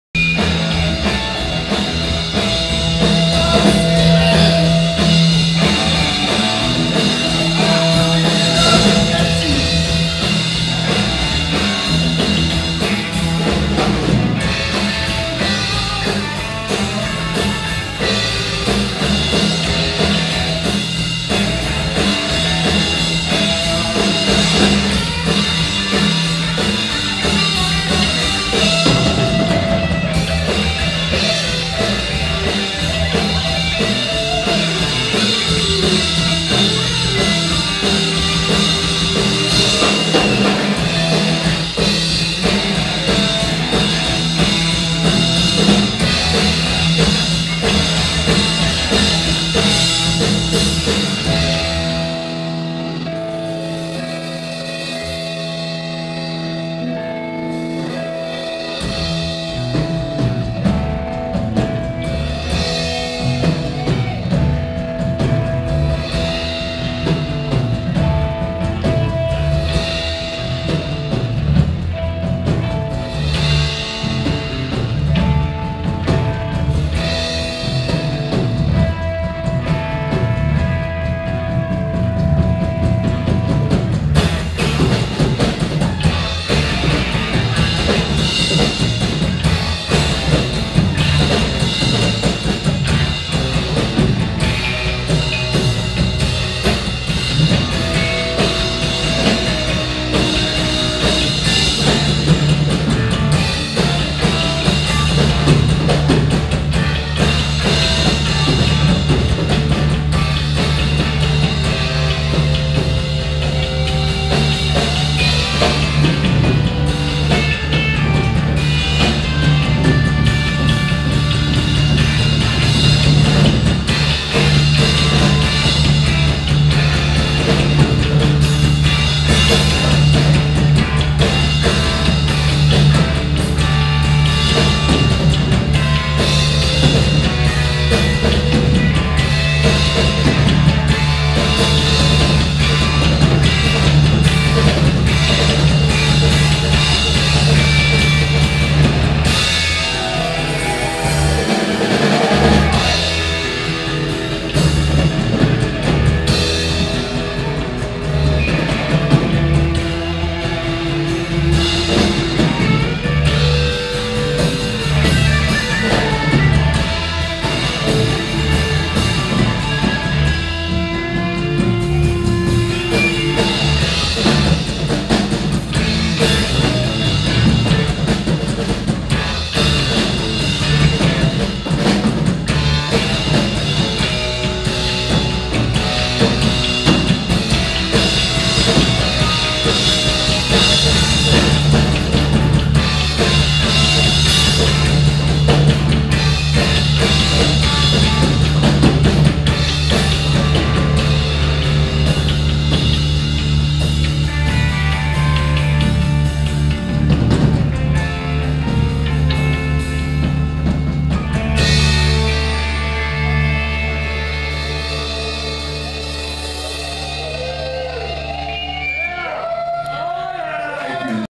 WHOligans June 2017
Three brothers and a killer bass comprise this band !!